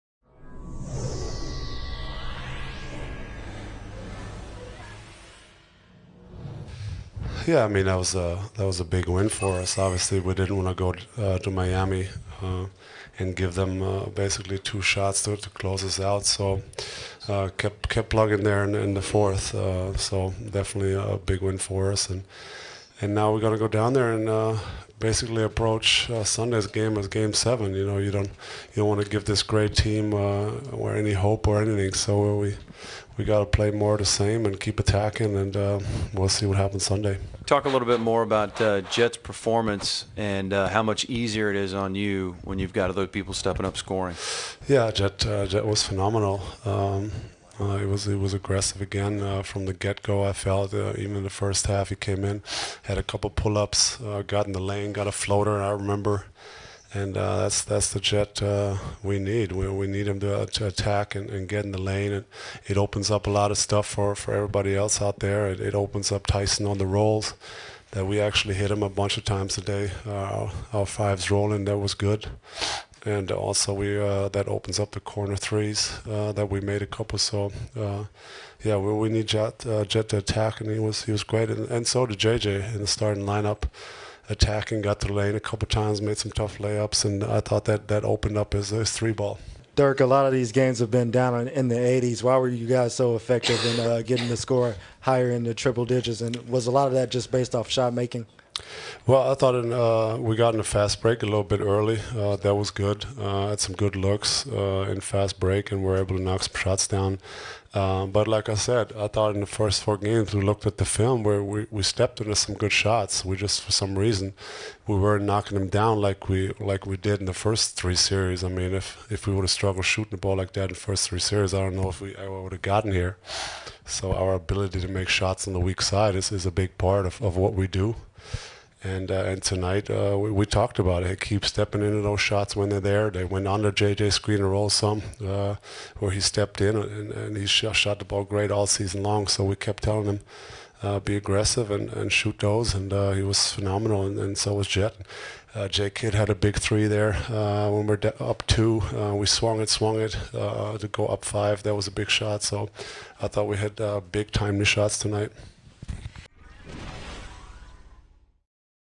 Post Game: Dirk Nowitzki
Dirk Nowitzki talks to the media about the Mavericks' big Game 5 win.